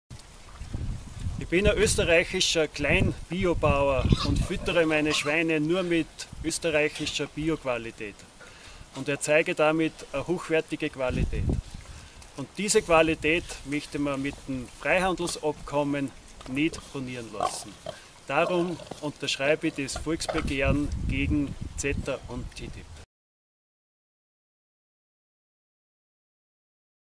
Charly, Biobauer, unterschreibt Volksbegehren gegen TTIP, CETA. TiSA
Charly, Biobauer aus Zwettl an der Rodl, unterschreibt am Gemeindeamt bis 30. Jänner 2017 das Volksbegehren gegen TTIP, CETA. TiSA.